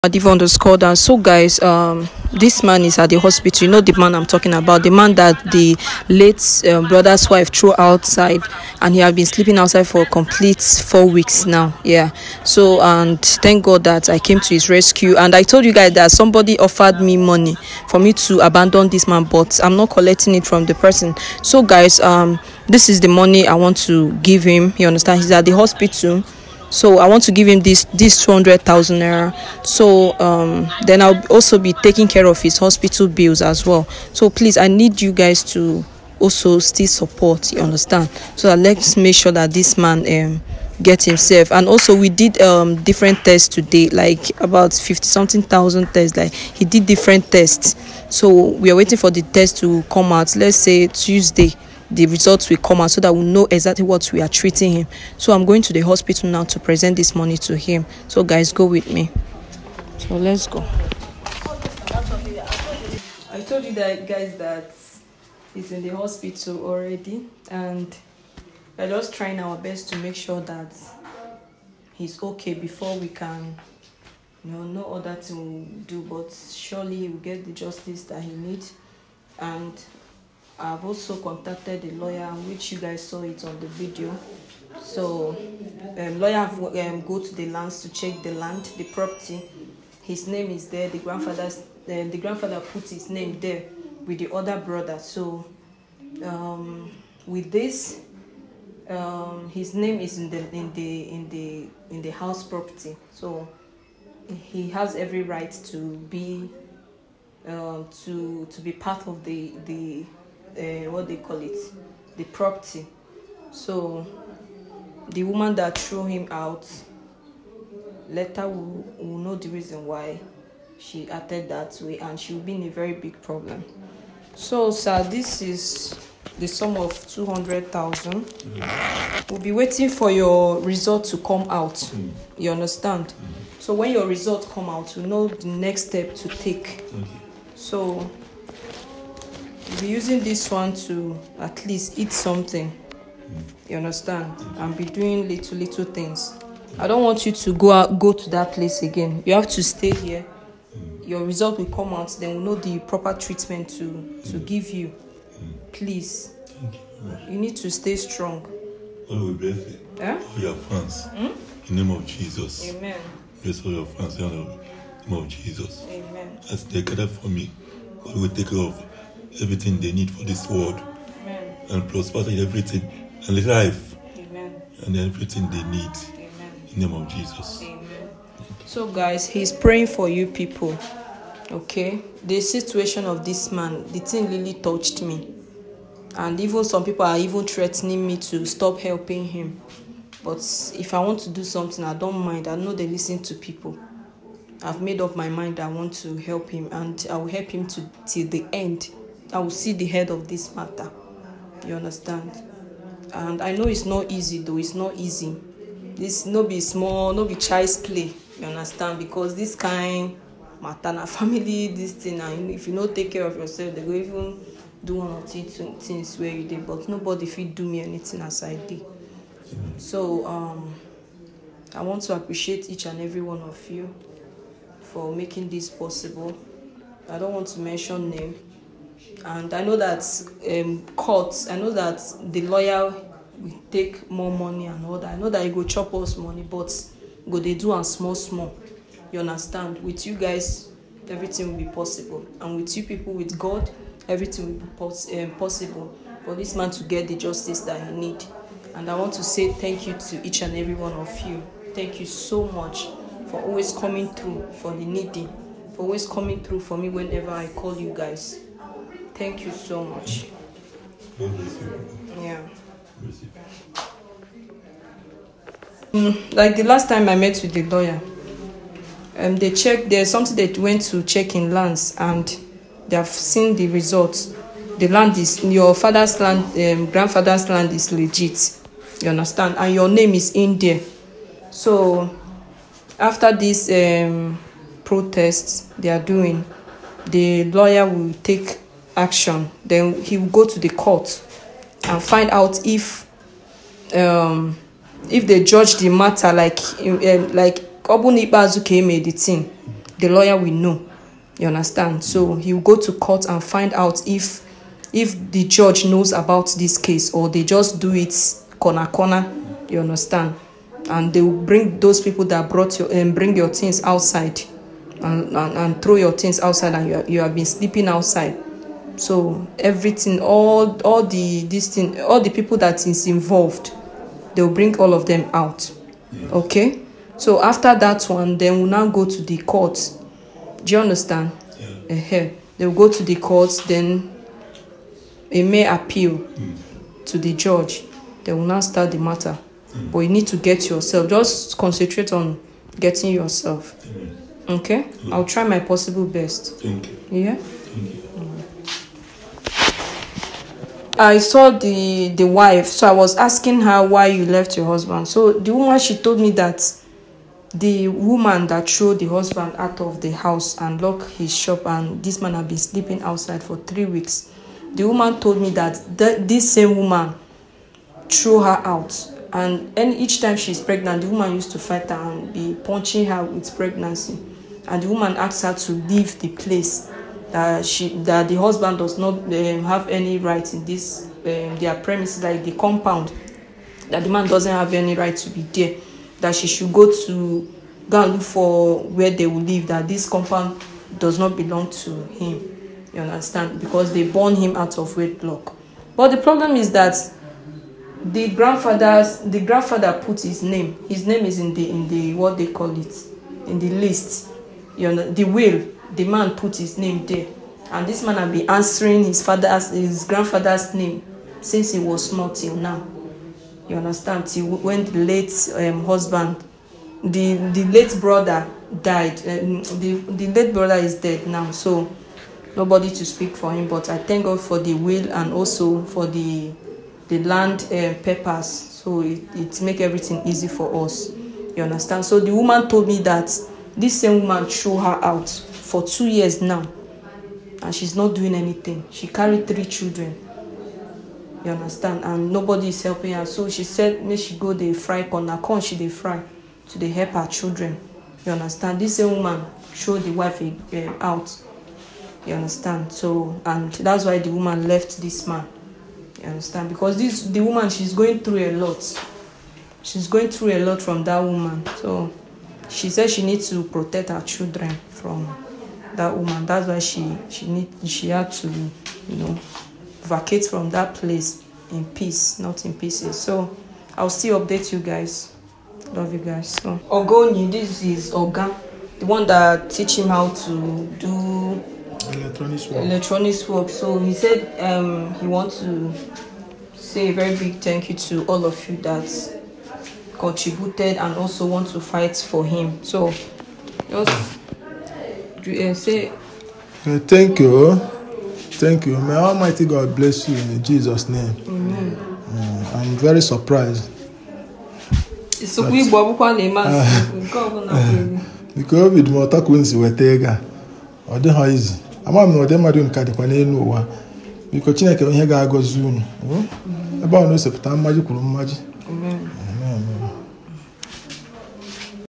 Another update about the man the late brother’s wife throw and he has been hospitalized cos of cold
Another update about the man the late brother’s wife throw out of his compound and his sick cos he slept outside for 3 weeks